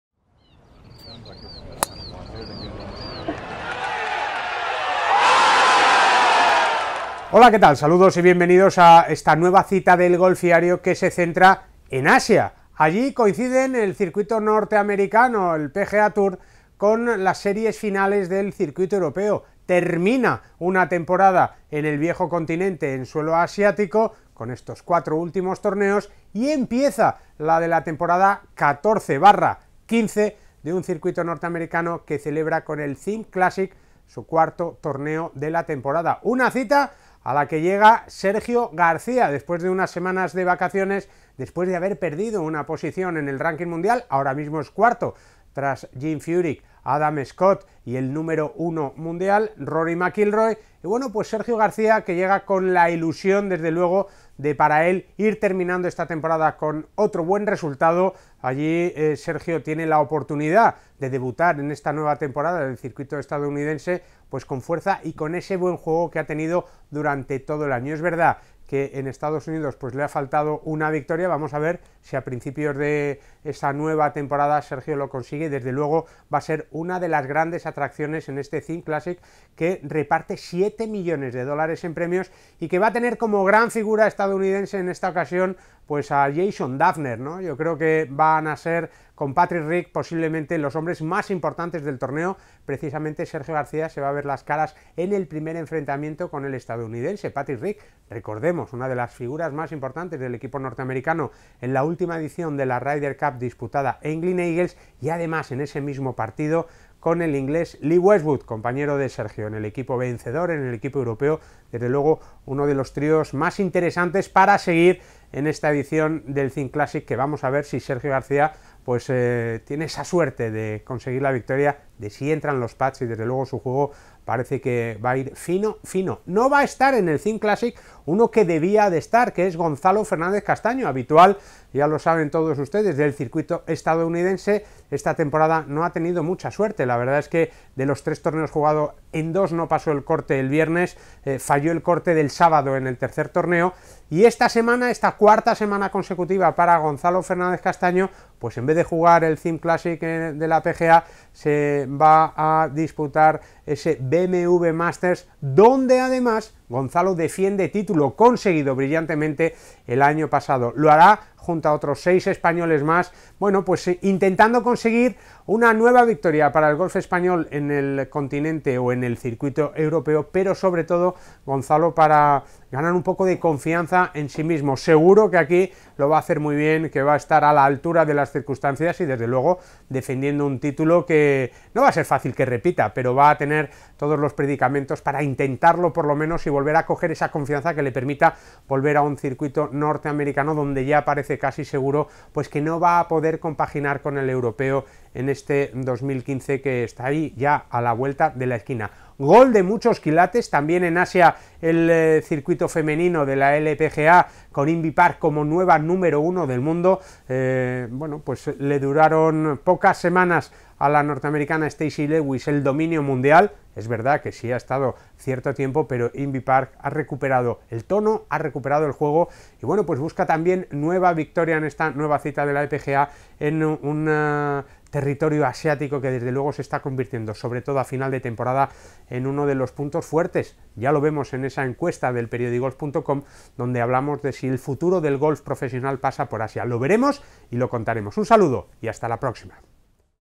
Sergio García y Fernández Castaño, protagonistas en Asia
Esta semana el golf mundial tiene su epicentro en Asia con la disputa de los torneos de la PGA, las Final Series del European Tour y del LPGA en tierras asiáticas. En los tres torneos habrá representación española pero serán, seguro, protagonistas en el CIMB Classic y en el BMW Masters Sergio García y Gonzalo Fernández Castaño.

El castellonés “debuta” en la PGA en la temporada 2014/2015 con la ilusión de empezar con buen pie aunque su reloj biológico sabe que estamos ya a final de temporada y que las fuerzas están ya justas. A pesar de eso, Sergio parte junto con Jason Dufner y Patrick Reed como uno de los grandes favoritos a la victoria y a subir algún puesto más en una clasificación mundial en la que es cuarto tras Furyk, Scott y McIlroy.

En el BMW Masters, Gonzalo Fernández Castaño defiende el título logrado el año pasado y llega con tres torneos jugados en la PGA con un balance muy pobre de dos cortes fallados y otro más el sábado. Esta semana tendrá, por lo tanto, una excelente oportunidad de mejorar en autoconfianza y juego.